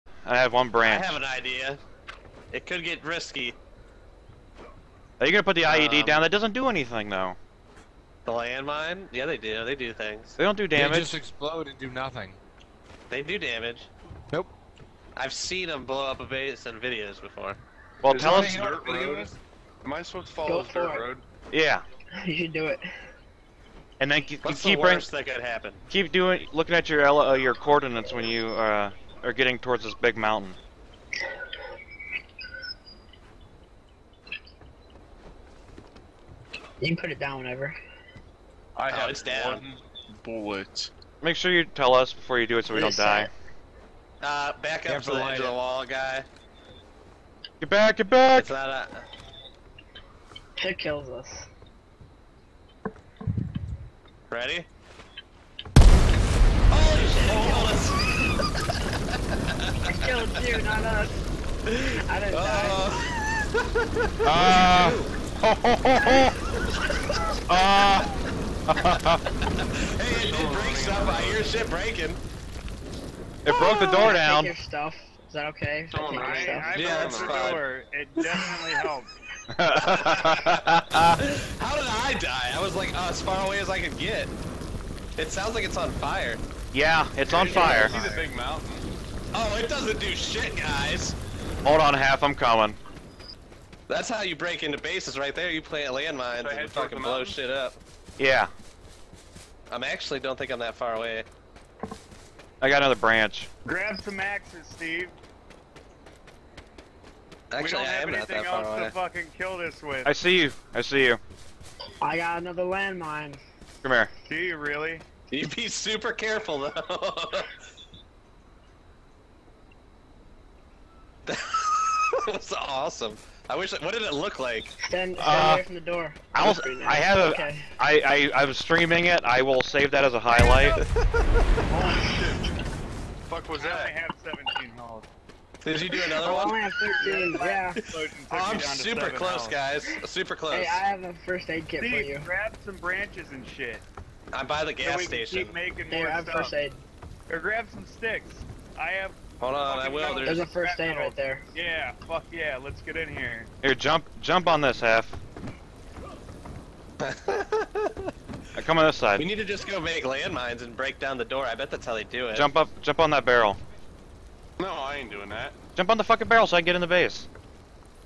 Steve Goes Boom
Steve Places a landmine down and soon regrets it!